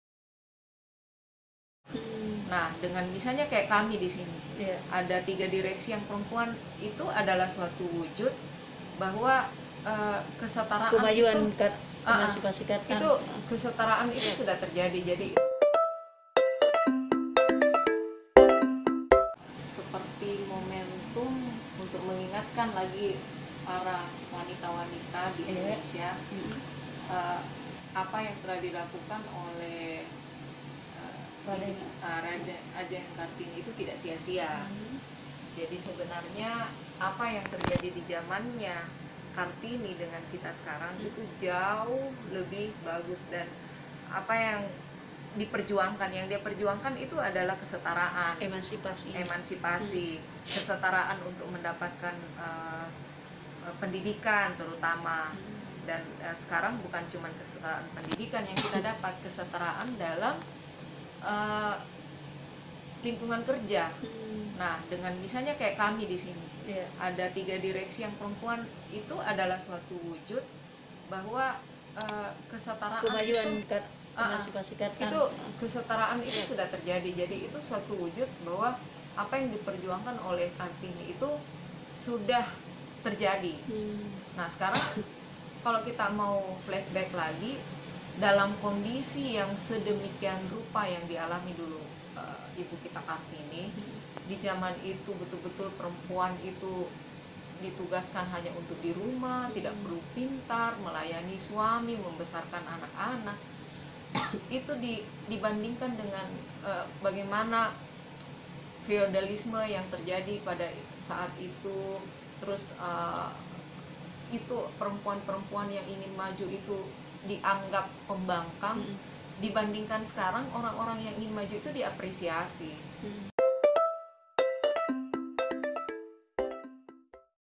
KartiniDay Bersama Direktur Bank Sulselbar, Irmayanti Sulthan
Apa yang sudah berubah sekarang, sejak Kartini memperjuangkan emansipasi wanita dahulu? simak wawancara terkini.id bersama Direktur Operasional dan IT Bank Sulselbar, Irmayanti Sulthan